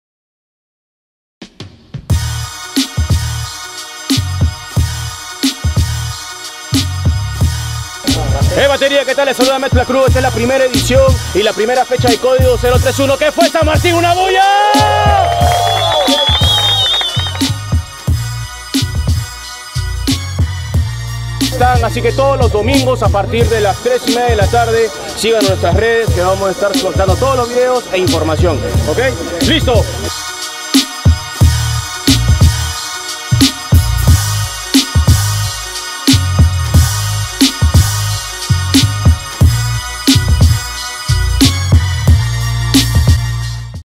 SOMOS C Ó D I G O - 0 3 1
Bienvenidos a Código 031 - el primer torneo semanal de improvisación y habilidad mental.

No te pierdas la siguiente fecha, están todos invitados a venir competir.
Estamos ubicados en Plaza Vizcardo y Guzmán - Av. Perú Cdra 35, Al frente del colegio IE Gran Amauta,  San Martín de Porres, Lima.

Host: Metz LaCrú

Beatmaker: Azerbeats (Chile)
Sigue su pagina y canal en Youtube hermano!

Jurados:
GEO - M4 - Franco FRZ

Cámara: Jimbs
Edición: C031

CÓDIGO 031 PERÚ by CODIGO.INT® - LIMA, PERÚ - MMIX -
Se reserva derecho de reproducción y/o duplicado del vídeo sin previa autorización de la misma.